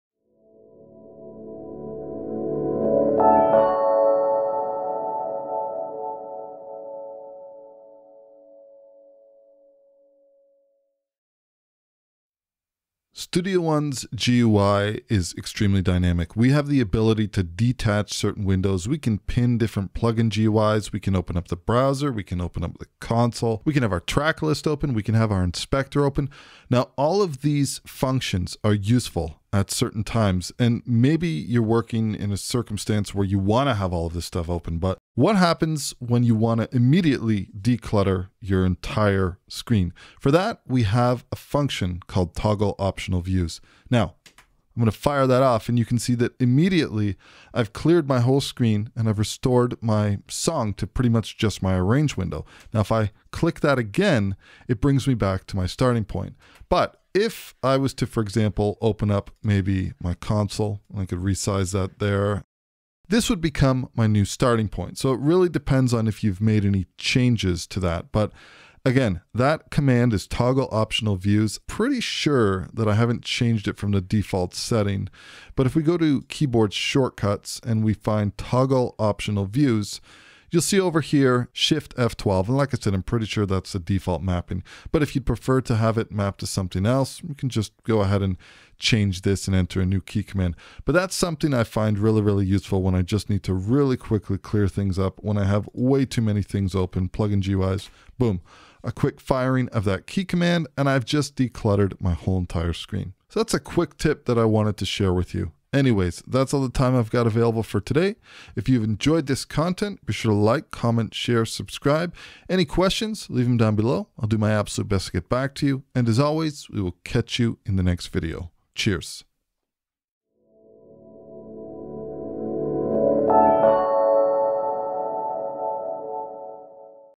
How To Toggle Optional Views In PreSonus Studio One
In this video, I demonstrate how to Toggle Optional Views in PreSonus Studio One 5. This simple but useful feature can be used to immediately clear any unnecessary windows, views, that are open in the GUI - with the added bonus of being able to bring them "back" into view just quickly.